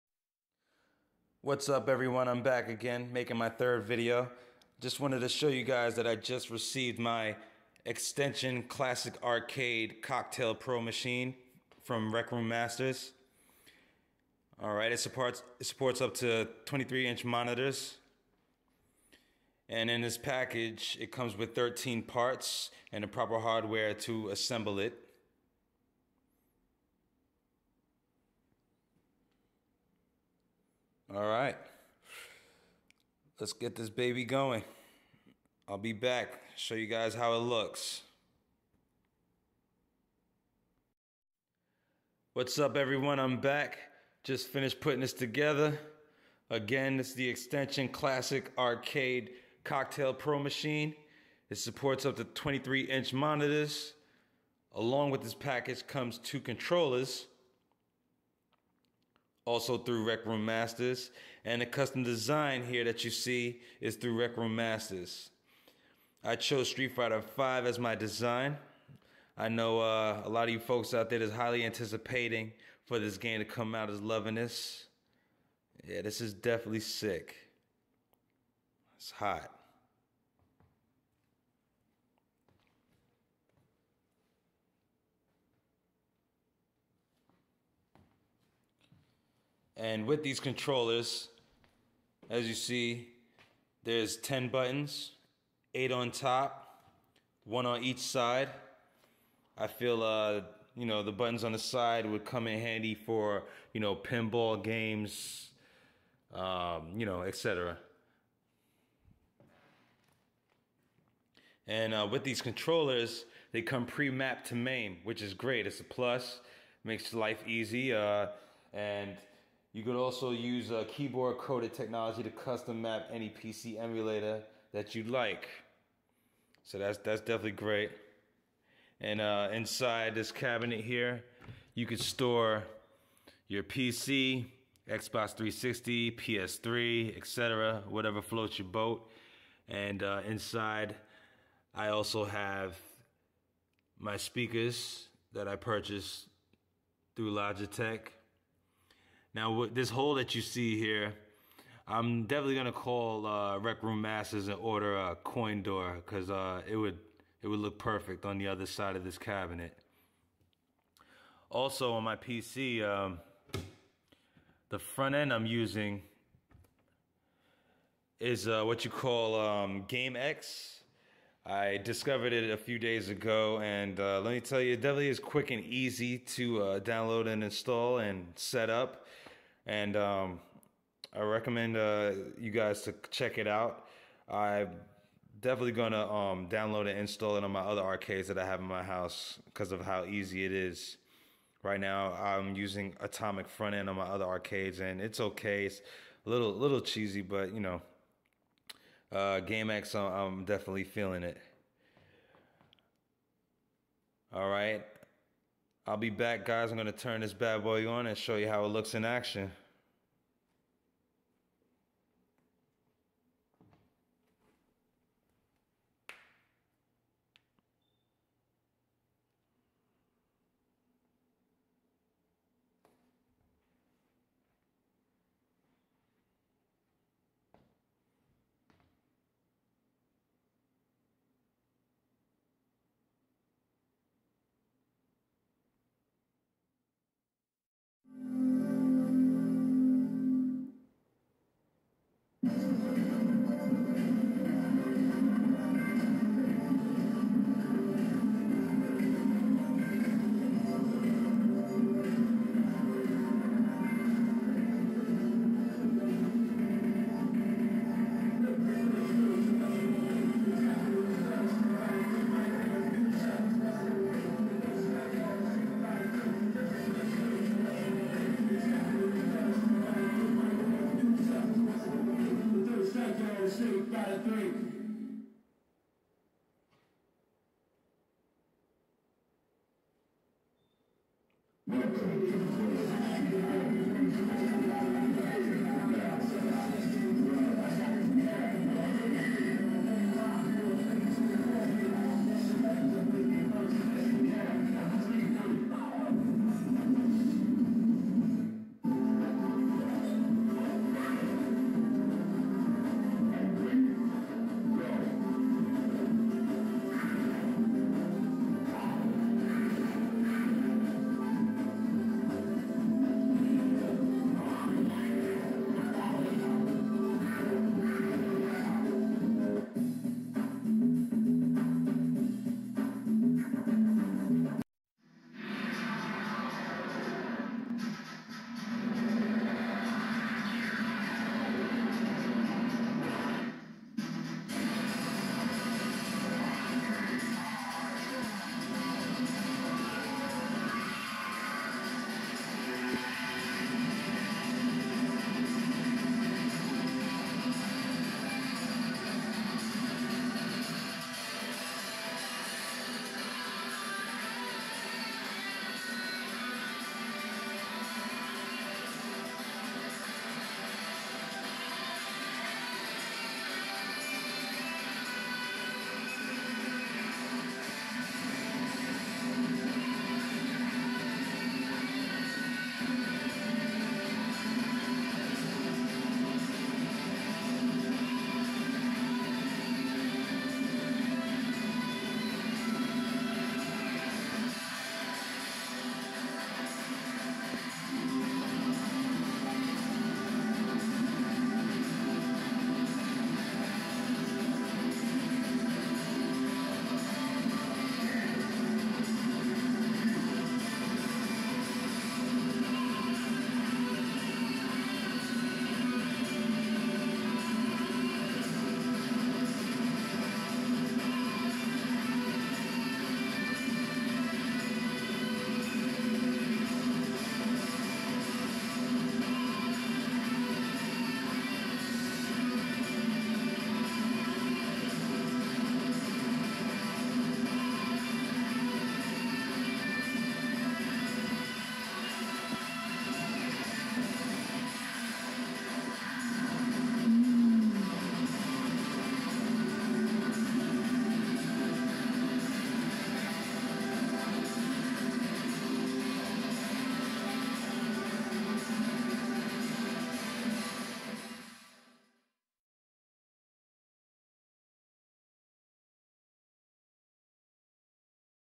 Street Fighter 5 Cocktail Arcade (Recroommasters)
(Lost Tapes) This video was back in April 2015. I purchased another arcade for my home and put it together.  It's the Xtension Classic Cocktail Arcade including a monitor mounting system. Supporting up to a 23" flat screen monitor and includes two controllers pre-configured for MAME emulators. Another cabinet from the amazing Recroommasters themselves.